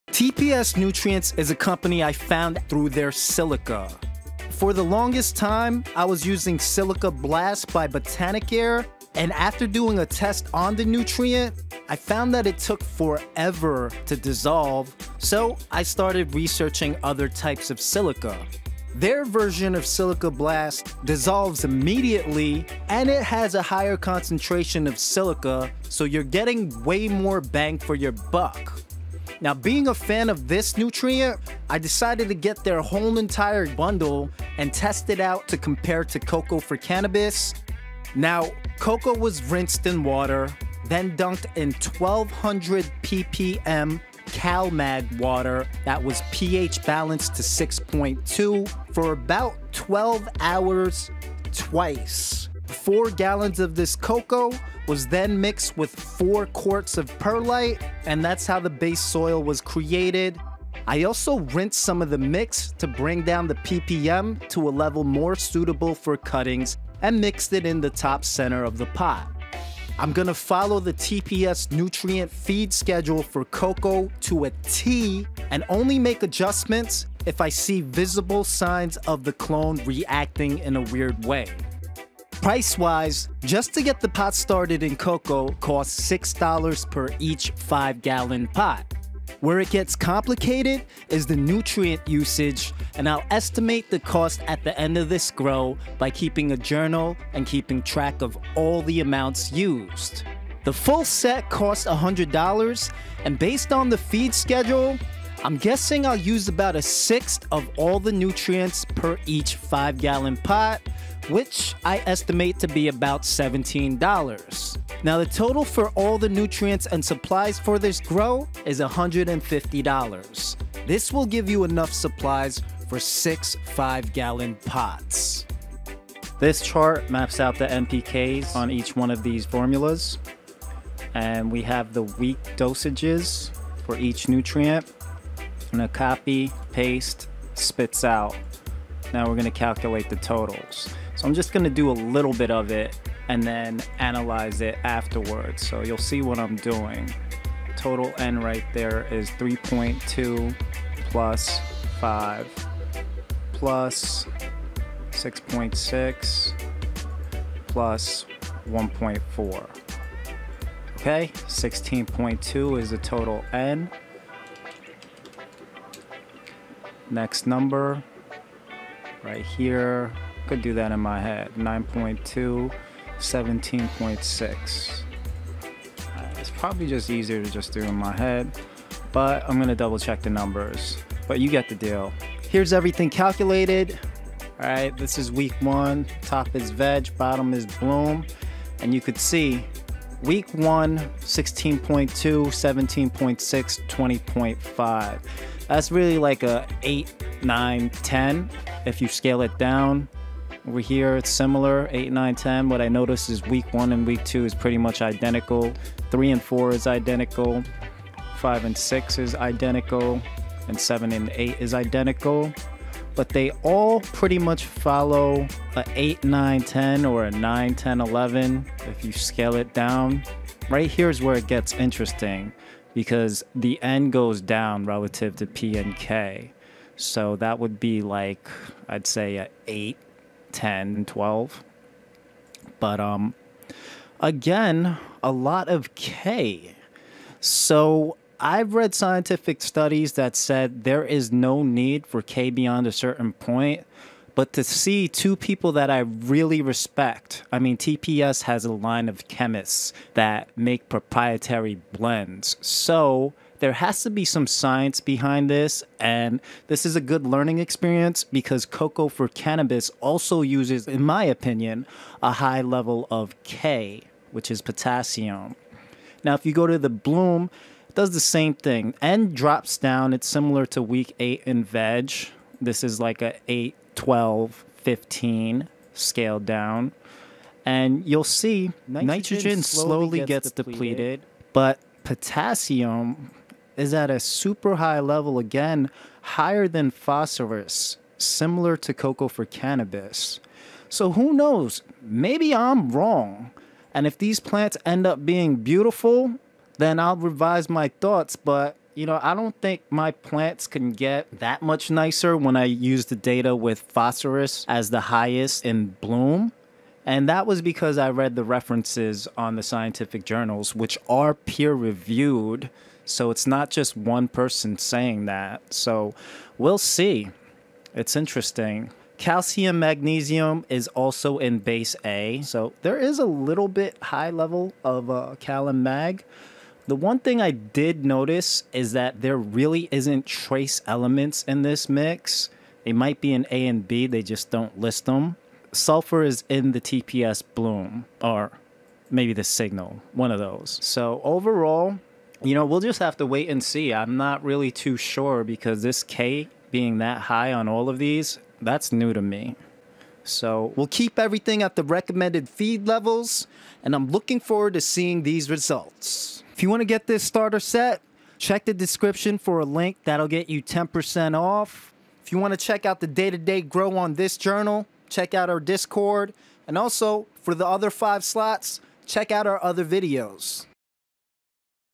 TPS Nutrients Coco Mix Breakdown - Meta Soil Cost Analysis and Preparation
TPS Nutrients is a proprietary blend of all the nutrients your weed or cannabis plants will need and this slot will attempt to make a fair comparison of it to the general hydroponics nutrients feed schedule recommended by coco for cannabis

I'll be putting these feed schedules head to head on top of comparing them to the 4 other soil slots in the meta soil project.

For MASTER LIST on Feed Schedule, Measurements, Supplies and Grow Equipment, goto

Timestamps:
Intro: 0:00
Mix and Preparation: 0:37
Price Analysis: 1:21
Lab NPK Analysis: 2:06

Coco Coir $24
10 lbs brick, used 1/7th the total so $3.42
CalMag $17
1 quart, used 30 ml which is approximately 1/30th or 56 cents
Perlite: $9
Used 1/4th or $2.25
Paint Strainer Bag

TOTAL NPK
VEG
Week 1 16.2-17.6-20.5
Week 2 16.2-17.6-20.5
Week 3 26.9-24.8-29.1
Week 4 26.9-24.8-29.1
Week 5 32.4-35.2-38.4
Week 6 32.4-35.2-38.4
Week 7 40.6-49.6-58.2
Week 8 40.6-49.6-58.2
BLOOM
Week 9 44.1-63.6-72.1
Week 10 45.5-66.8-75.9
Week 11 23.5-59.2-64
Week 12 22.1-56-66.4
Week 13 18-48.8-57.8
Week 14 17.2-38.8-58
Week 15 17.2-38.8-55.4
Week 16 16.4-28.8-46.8

Current Tent: (Will change)
Current Light (Will change)
Current Fan
Inline Fan Kit
Carbon Filter